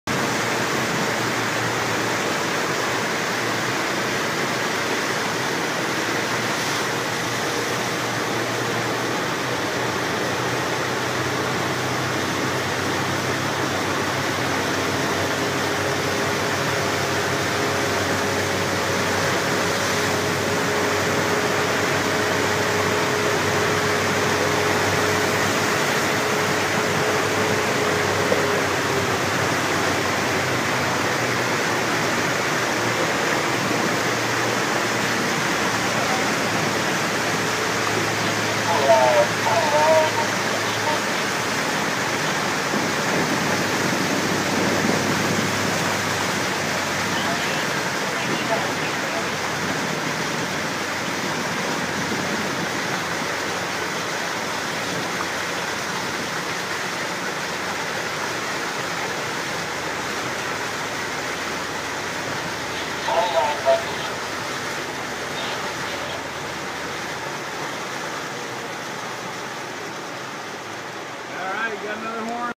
Empty Lake Powell, Utah
On September 23rd, Pavan and I visited Lake Powell on the cruise. It’s a man-made reservoir on the Colorado River in Utah and Arizona and peak season just passed three weeks back. Very peaceful lake.